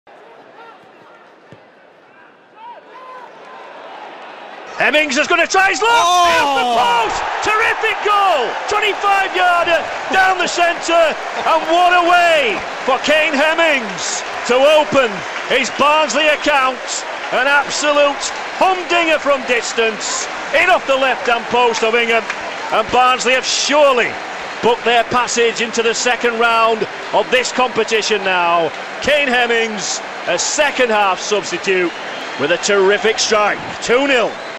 Fantastic debut goal for Barnsley FC's Kane Hemmings
Kane Hemmings scored a fantastic first goal for Barnsley in the Johnstone's Paint Trophy against York City

Tuesday 2nd September 2014